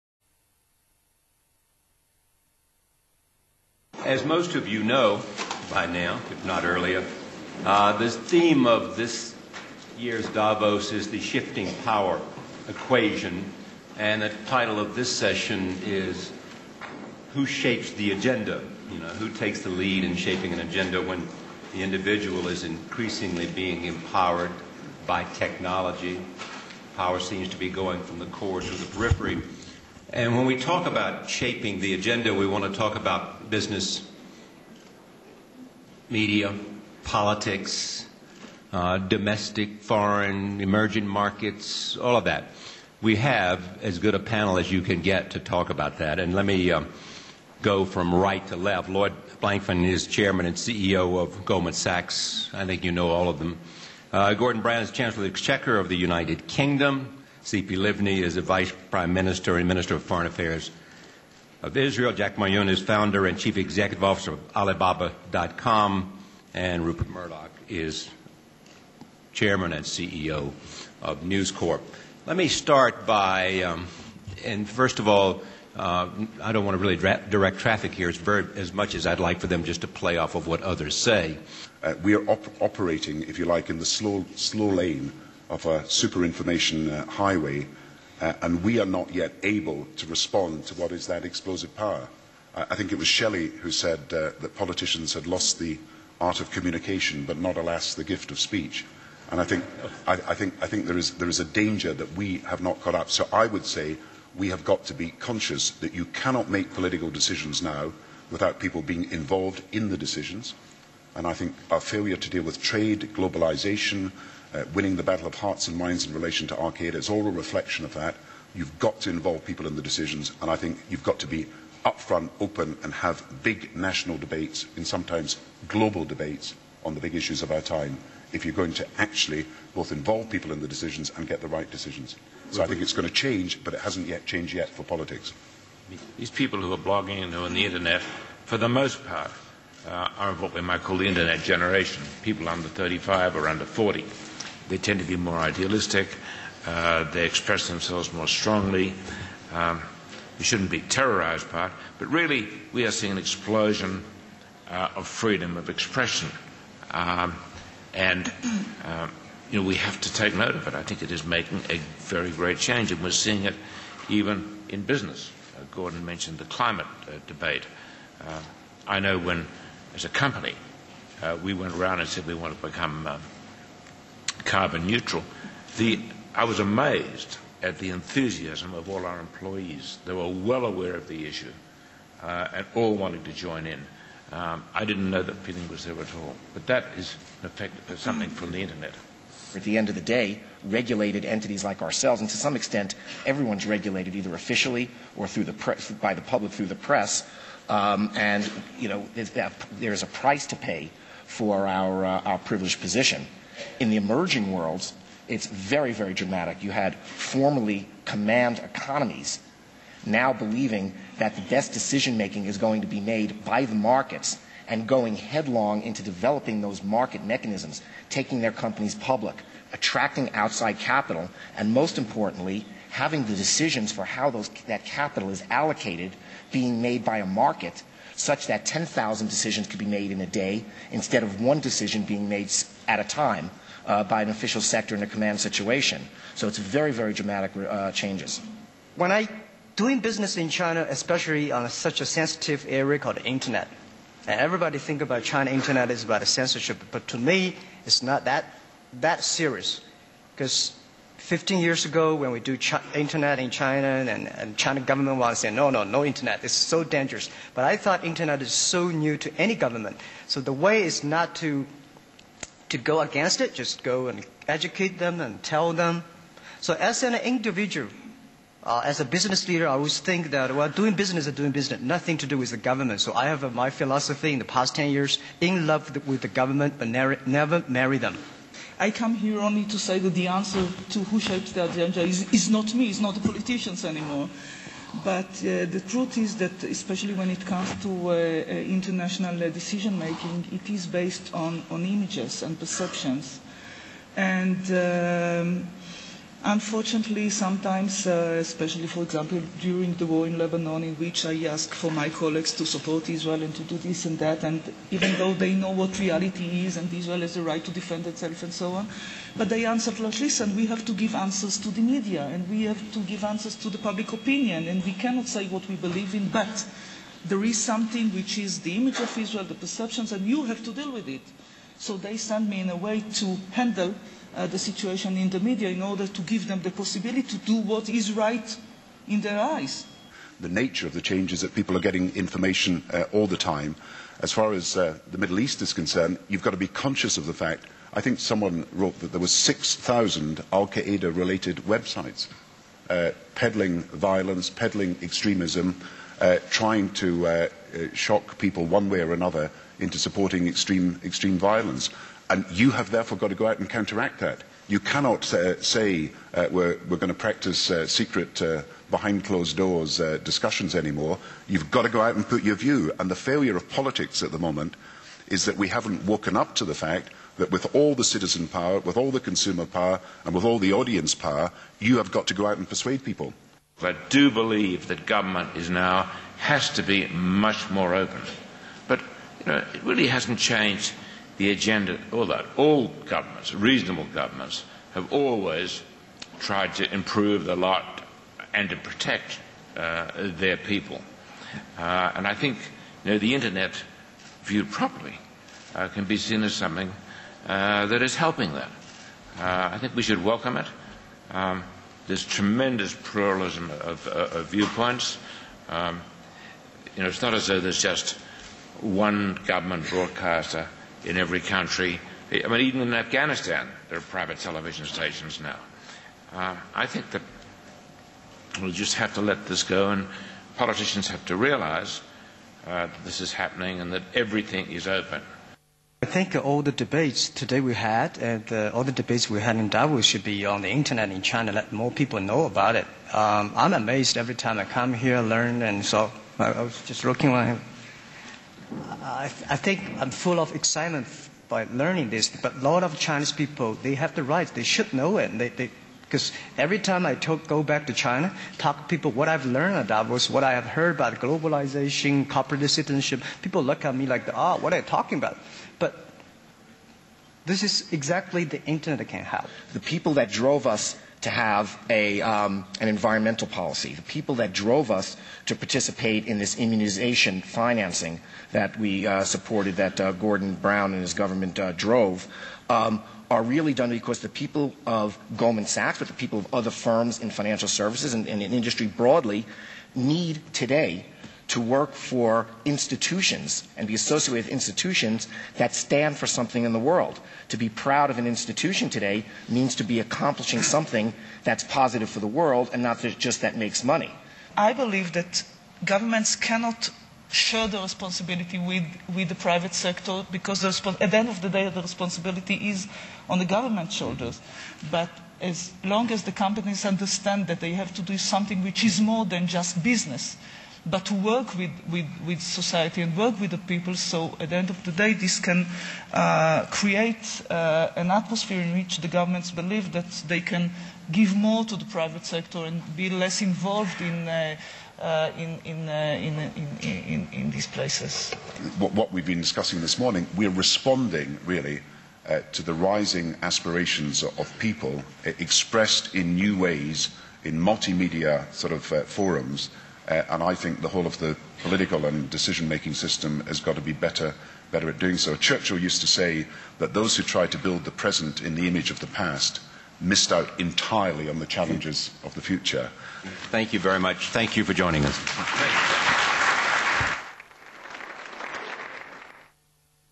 Davos Annual Meeting 2007 - Who Will Shape the Agenda?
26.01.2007

Who Will Shape the Agenda?

The global economy is in the midst of a historic shift. The integration of economies and markets around the world impacts the work of every significant actor - from government to the media to business. However, managing the balance of power and co-dependencies between the three can in large part define the strength and health of a modern democracy.
1) How are the responsibilities of government, the media and business leaders shifting?
2) How can they work more effectively - both together and separately?
3) Who will take the lead in shaping the agenda when the individual is increasingly being empowered due to changes in technology?
Lloyd C. Blankfein, Chairman and Chief Executive Officer, Goldman Sachs Group, USA
Gordon Brown, Chancellor of the Exchequer of the United Kingdom
Tzipi Livni, Vice-Prime Minister and Minister of Foreign Affairs of Israel
Jack Ma Yun, Founder and Chief Executive Officer, Alibaba.com, People's Republic of China; Young Global Leader
Rupert Murdoch, Chairman and Chief Executive Officer, News Corporation, USA
Chaired by
Charlie Rose, Anchor, Executive Producer and Executive Editor, The Charlie Rose Show, USA